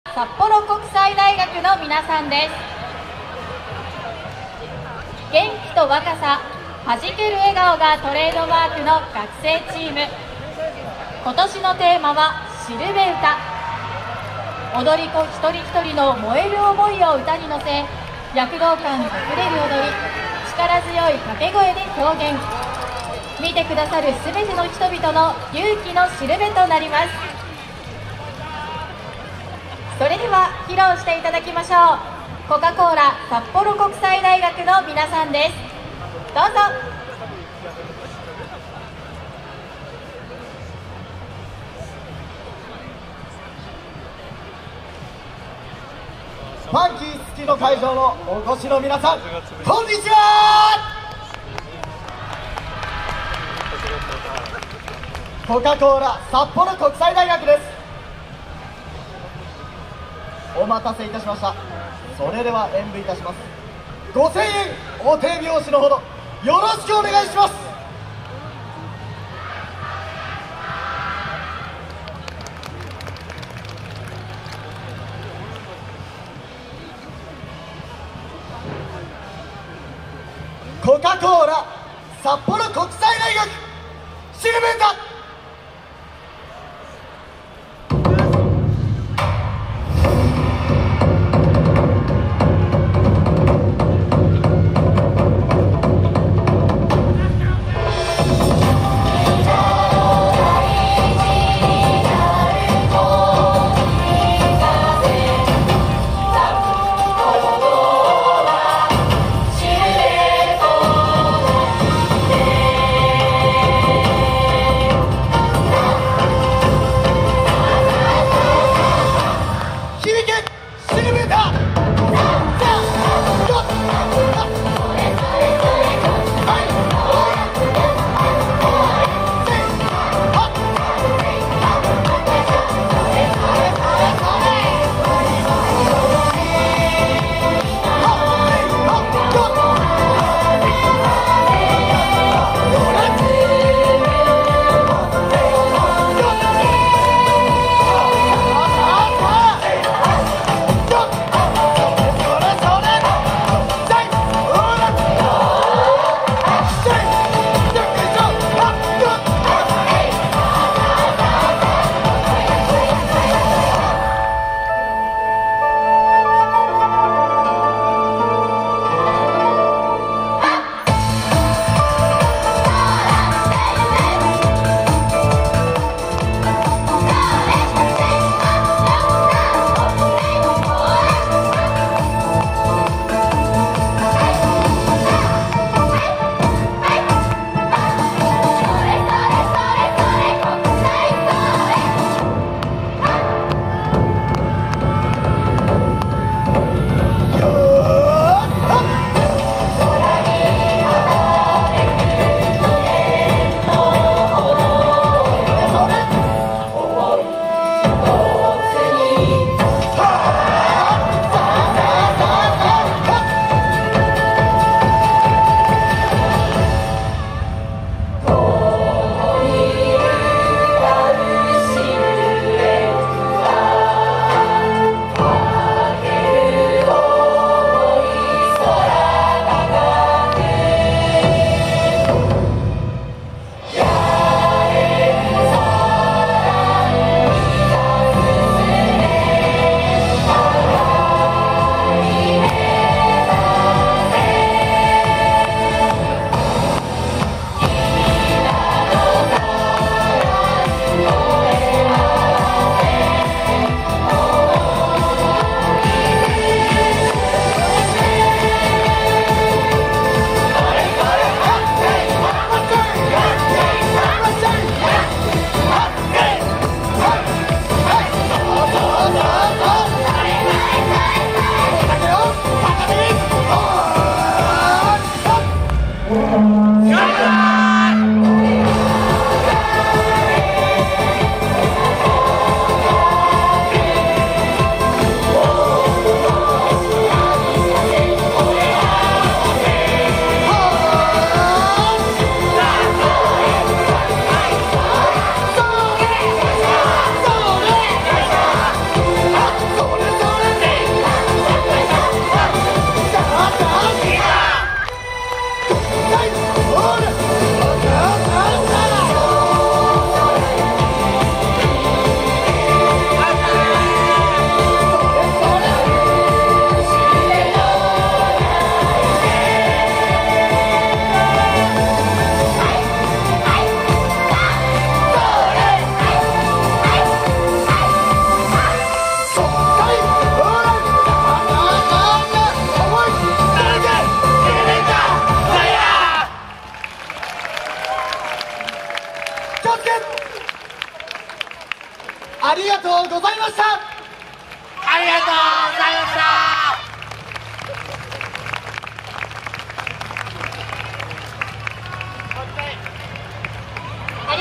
コカ・コーラ札幌国際大学【4K_full_100Mbps】／7日／第33回YOSAKOIソーラン祭り2024
2024/06/07（令和6年6月7日）
会場：FUNKY!すすきの会場　サッポロガーデンパーク会場　道庁赤れんが会場　カナモトホール